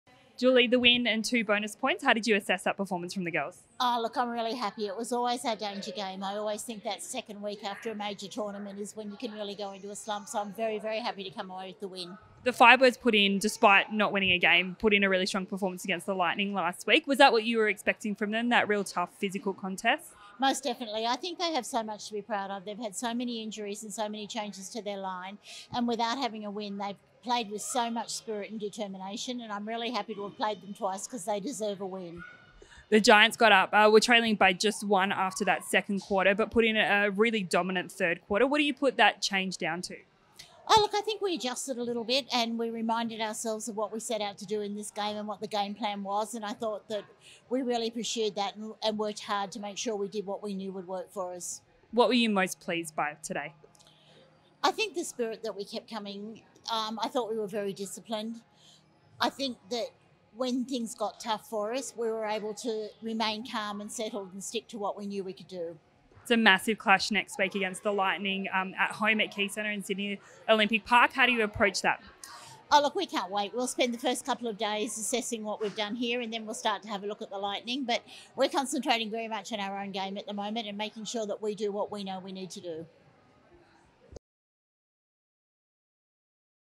Round 11 Post Match Julie Fitzgerald
GIANTS Netball coach Julie Fitzgerald speaks after the win over the Firebirds in round 11 in Canberra.